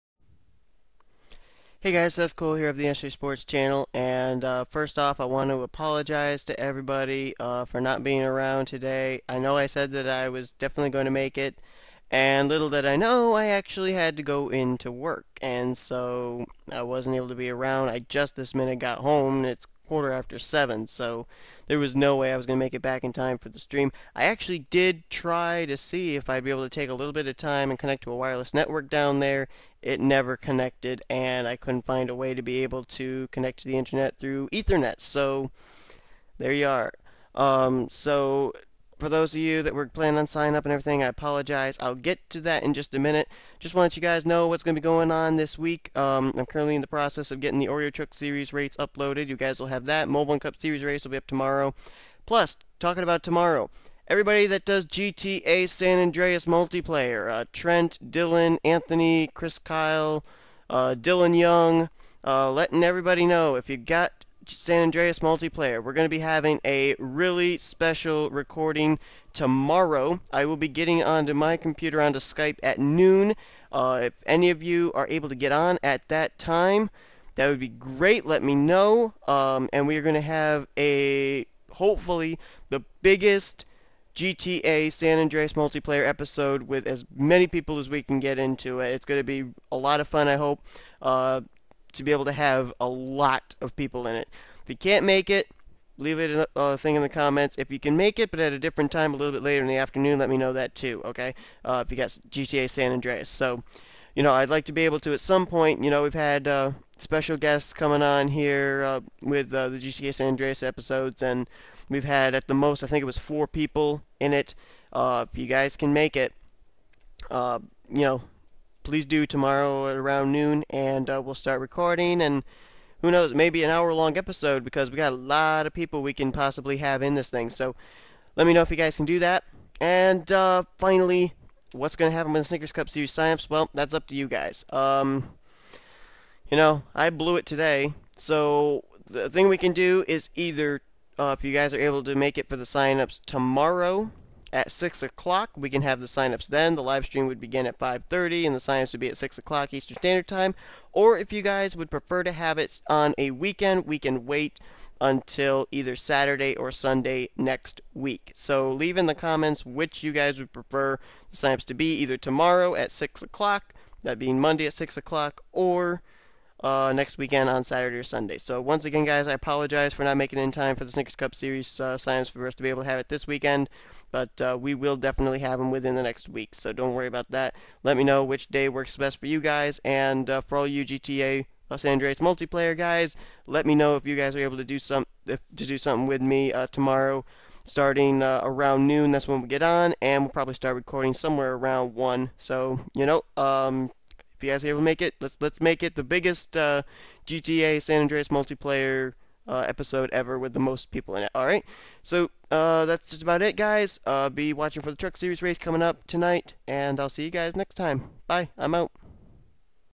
A few announcements GTA SAN ANDREAS PLAYERS PLEASE WATCH!!!
Update #1: GTA San Andreas Biggest Recording Tomorrow!!!
Update #2: Snickers Cup Series Season 6 Signups Rescheduling.